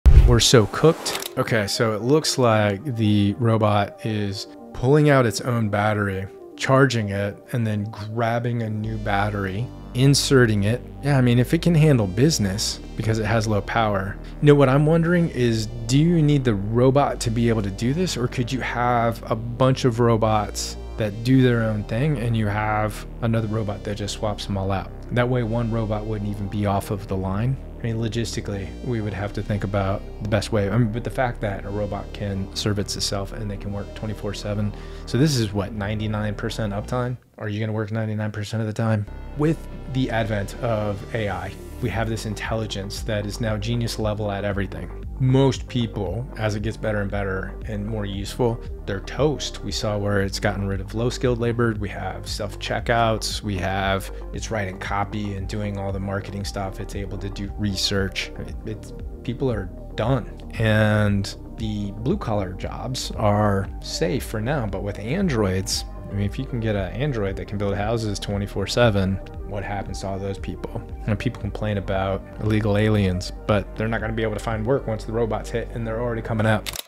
Robots That Never Rest Are Here
A humanoid robot just swapped its own battery—no downtime, no human needed. That means:
✅ 99% uptime
✅ 24/7 productivity
✅ Zero breaks, zero burnout

This is the leap that turns robots from tools into an unstoppable workforce.
The question is: what happens to us when machines never need to rest?